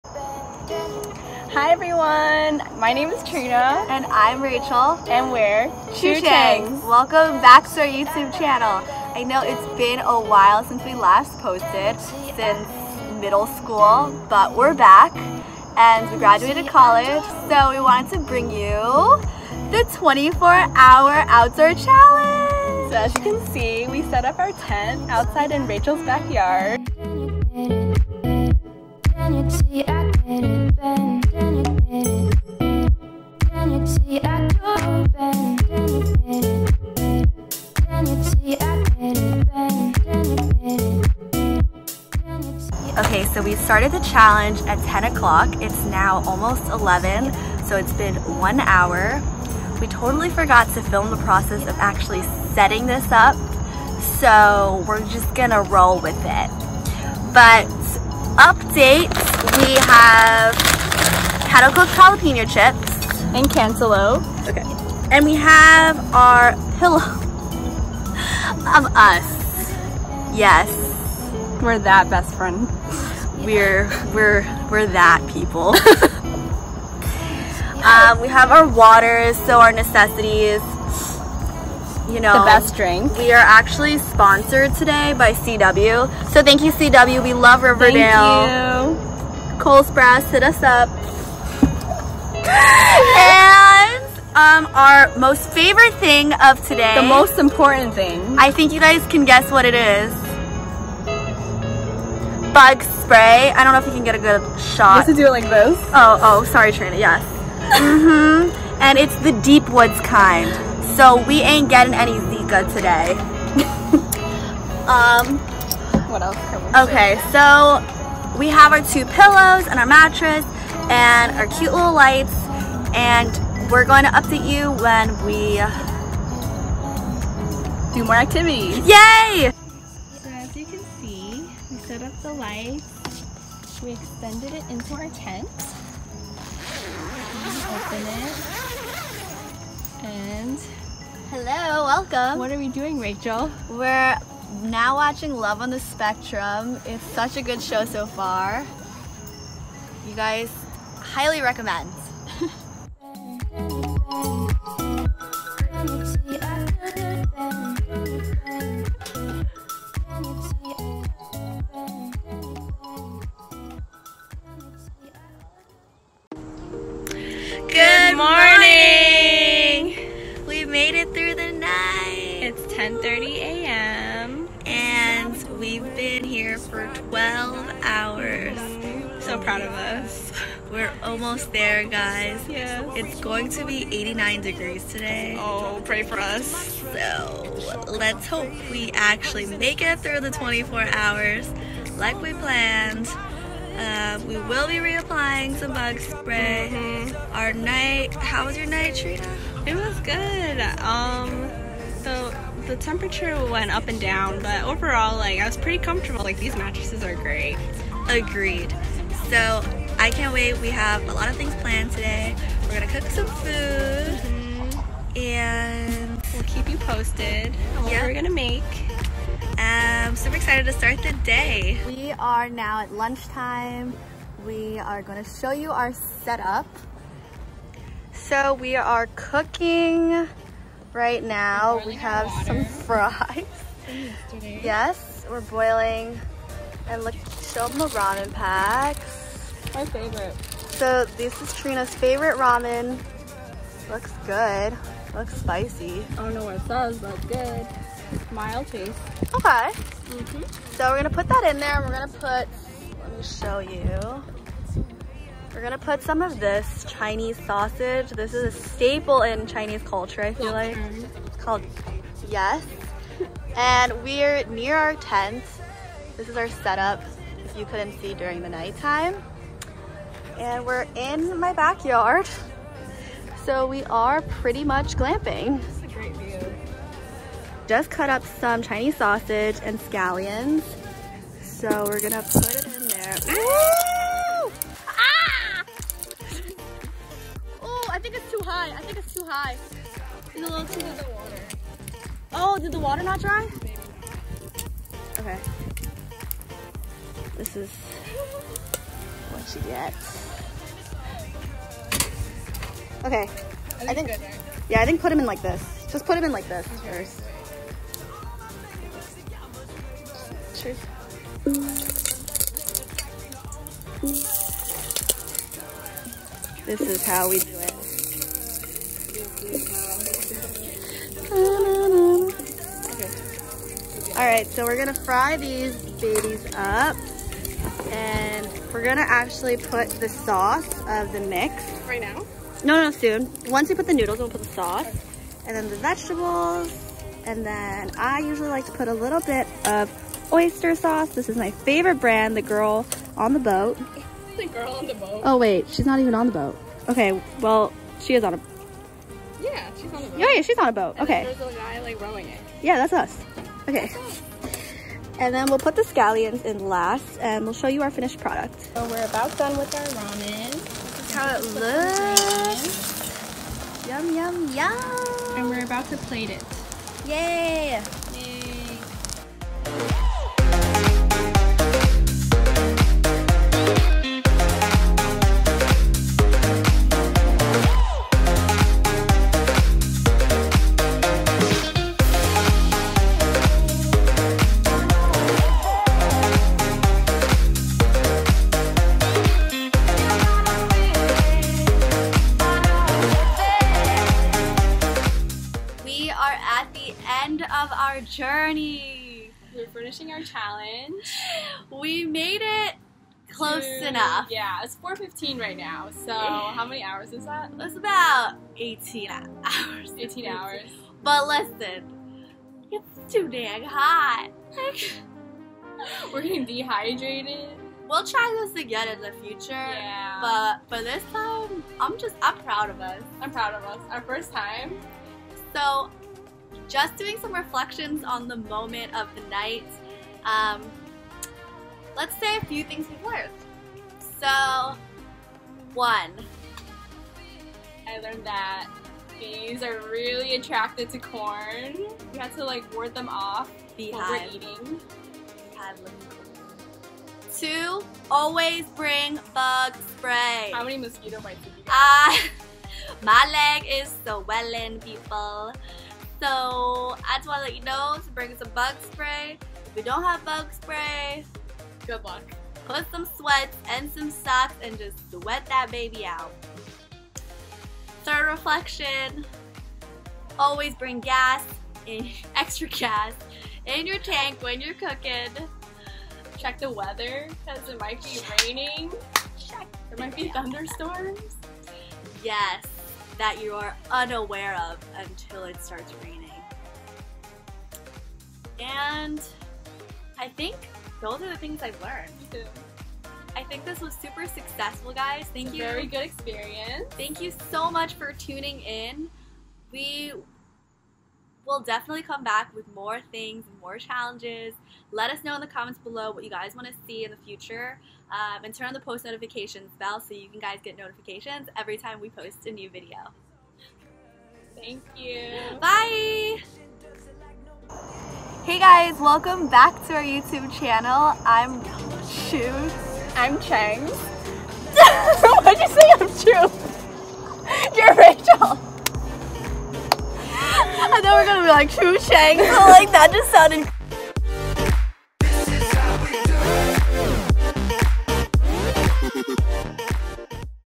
24 HOUR OVERNIGHT BACKYARD CAMPING CHALLENGE
We decided to attempt the 24 hour outdoor camping challenge!! Tune in to see if we were successful.

**All songs used are NOT OURS**